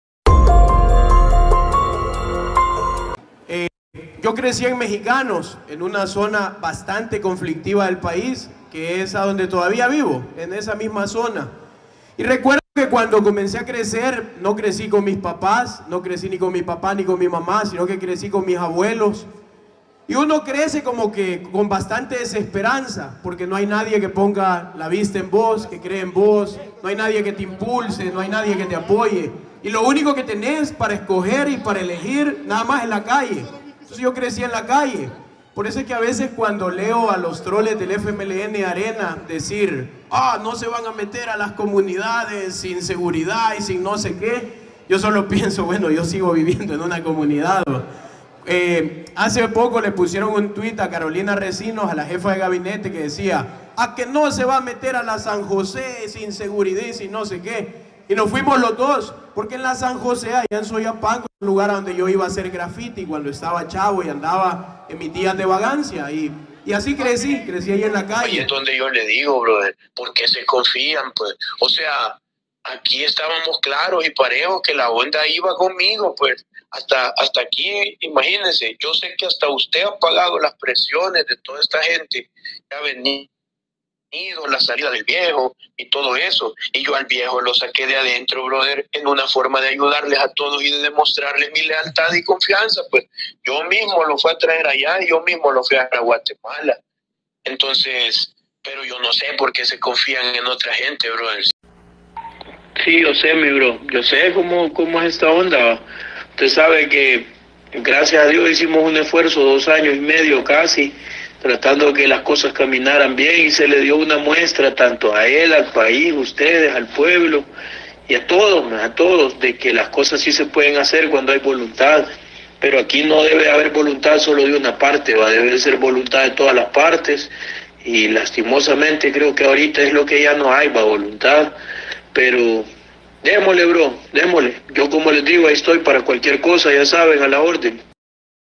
Carlos Marroquin Dice que Creció en un Barrio de Pandillas en mejicanos
carlos marroquin dice que creció en barrio de pandillas en mejicanos